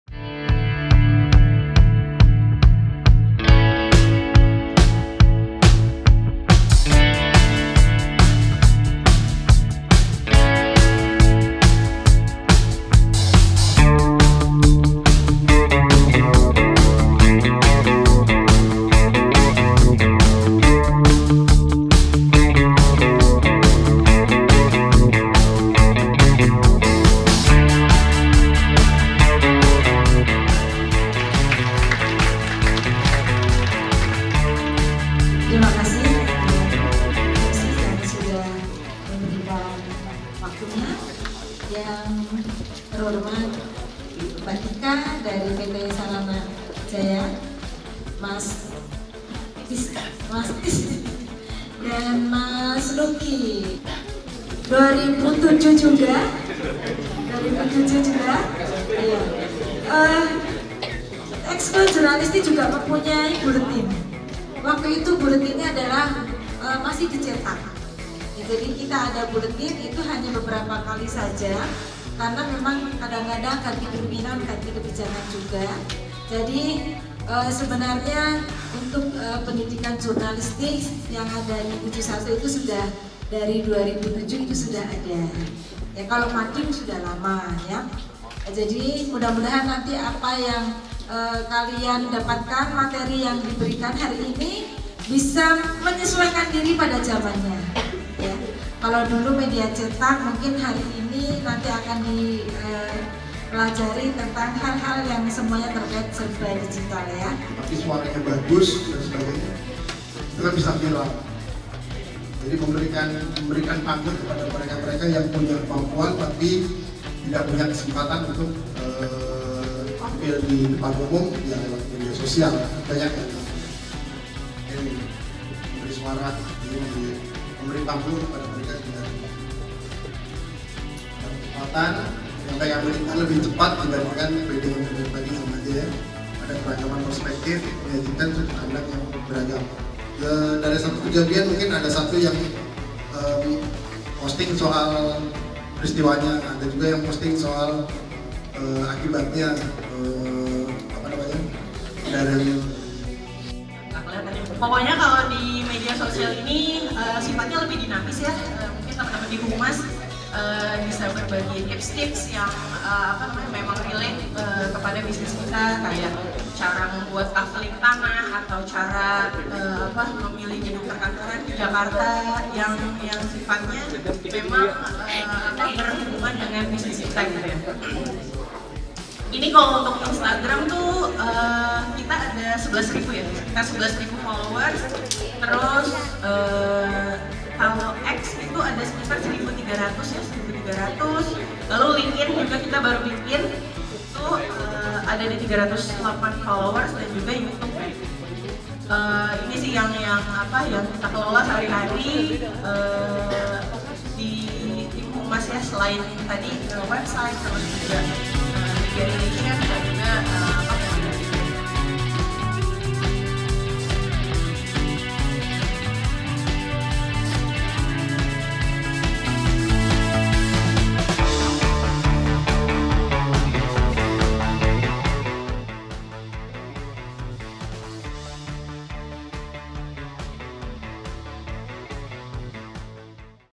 Belajar Jurnalistik hingga Konten Edukatif | Warta Kota Mengajar di SMAN 71 Jakarta
WARTAKOTALIVE.COM, DUREN SAWIT — Keseruan nampak dari wajah para siswa yang mengikuti kegiatan Warta Kota Mengajar di SMAN 71 Jakarta, Rabu (28/8/2024).